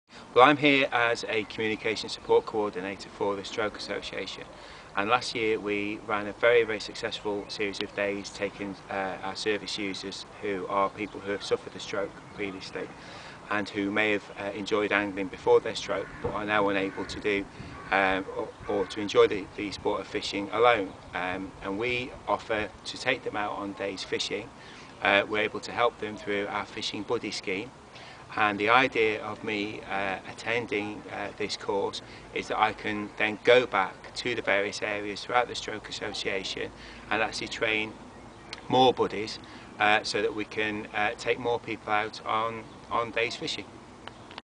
Dreamstore Academy delivers for the Stroke Association
Andy Biggar of the Stroke Association explains what the SQA accredited and UKCC standard Dreamstore Academy Level 2 will mean to the Stroke Association in the North West of England.